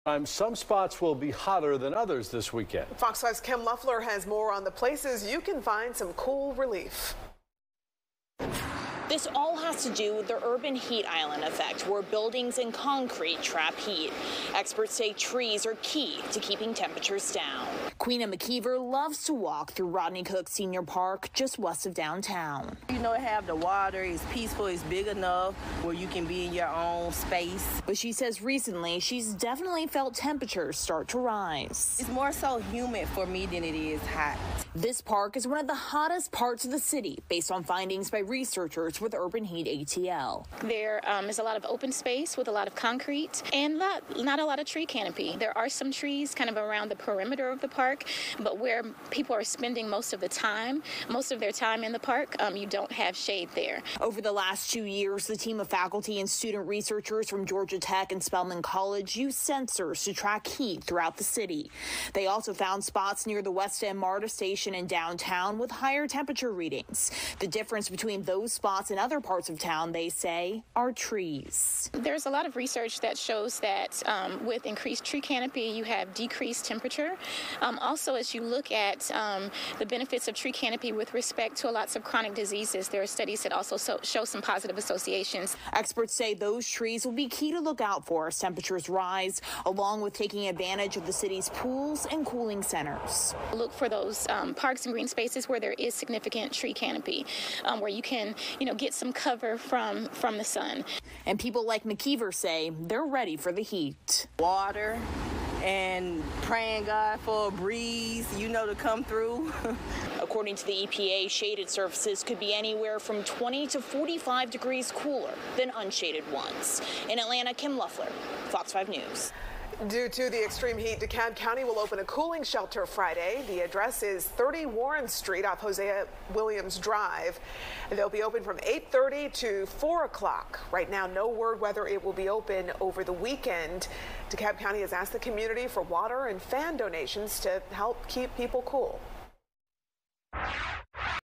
Tracking the heat in Atlanta | FOX 5 News
Over the last two years, a team of faculty and student researchers from Georgia Tech and Spelman College used sensors to track heat throughout the city.